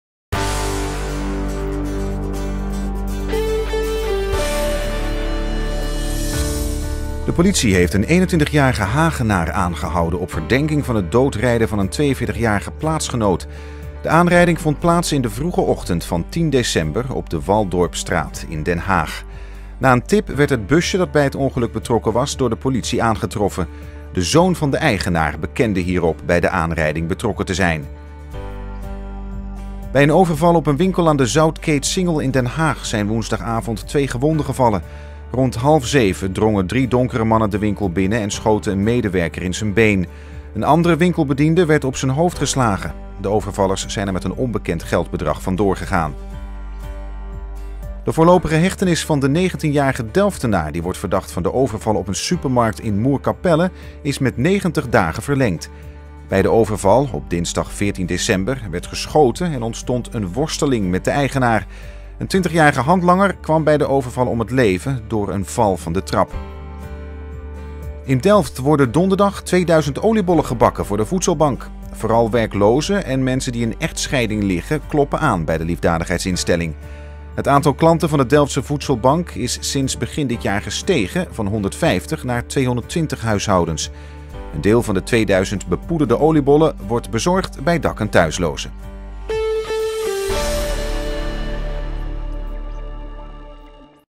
West in 90 seconden, donderdag 30 december 2010 - Westonline.nl
Onderwerpen:

* Bestuurder dodelijke aanrijding Waldorpstraat aangehouden
* Twee gewonden bij overval Schilderswijk
* Overvaller Moerkapelle langer vast
* Oliebollen voor voedselbank Delft